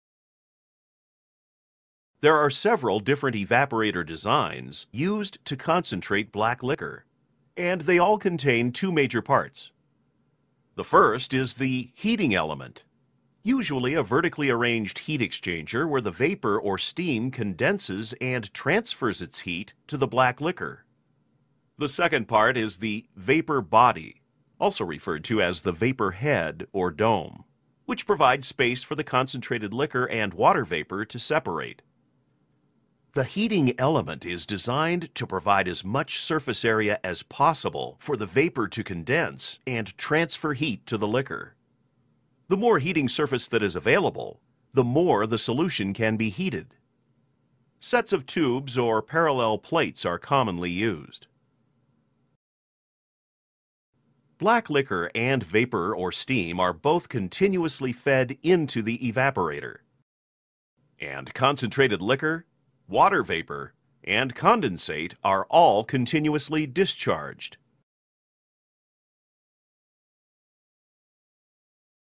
Black Liquor Evaporators - Equipment
to view the full video and purchase access to our other Pulping courses.

The Black Liquor Evaporators - Equipment course describes several different evaporator designs that have been used to concentrate black liquor. These include rising film evaporators, falling film evaporators, falling film concentrators, and forced circulation concentrators. It also contains discussions on scaling and boil outs, the role of the surface condenser, overall driving force and capacity, and the removal of non-condensable gases from a multiple effect evaporator set.